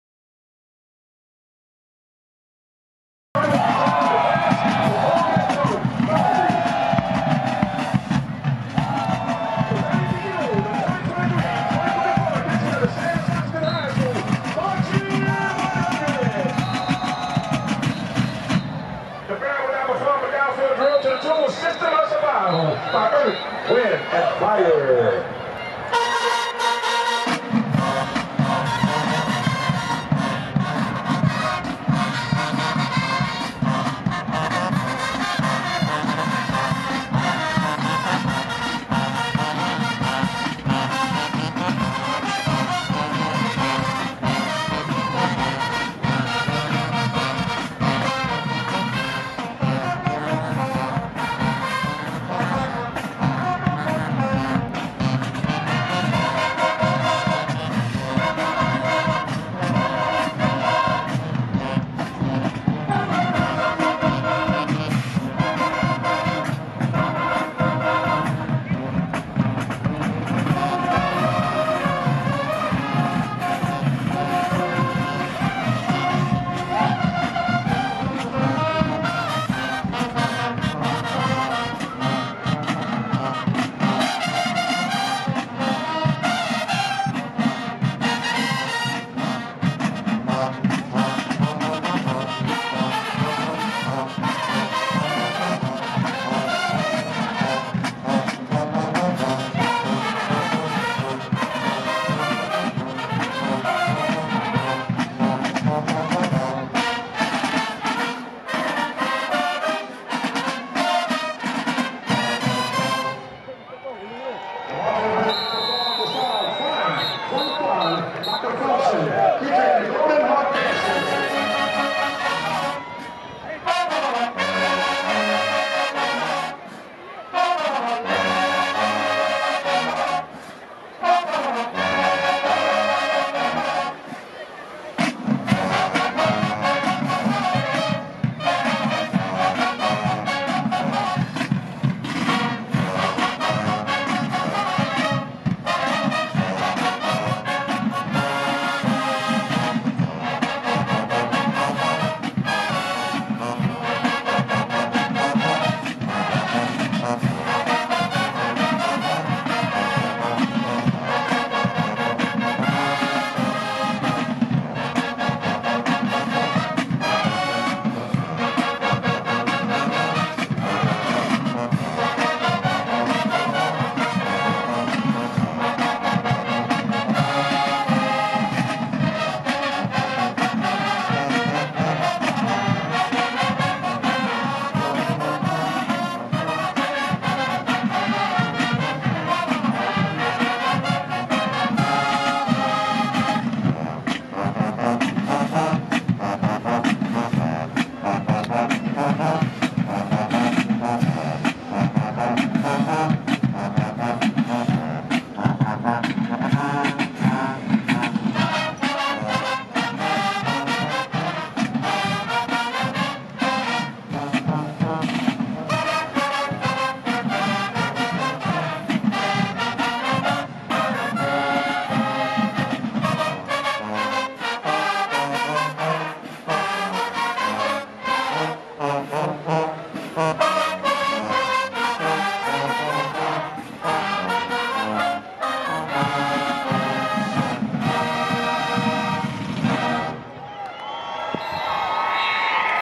St Aug halftime show performance 2023 @ Edna Karr game (HD) 4K
Official game highlights featuring the St Aug marching band performance during halftime. Highlighting all the marching groups this season. Be sure to watch in HD Format